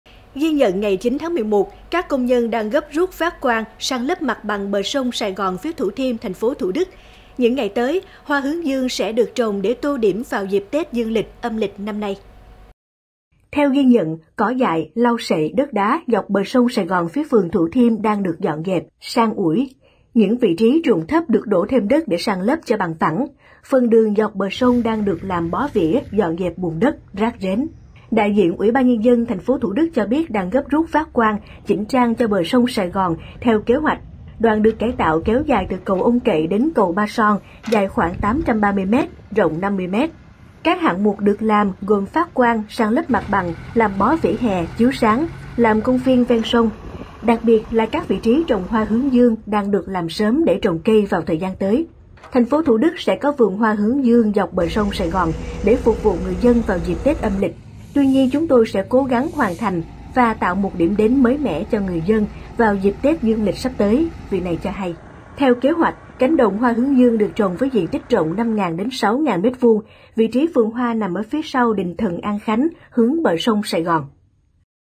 Bờ sông Sài Gòn phía Thủ Thiêm đang được dọn dẹp bể trồng hoa hướng dương
Các công nhân đang gấp rút phát quang, san lấp mặt bằng bờ sông Sài Gòn phía Thủ Thiêm, TP Thủ Đức. Những ngày tới hoa hướng dương sẽ được trồng để tô điểm vào dịp Tết dương lịch, Tết âm lịch năm nay.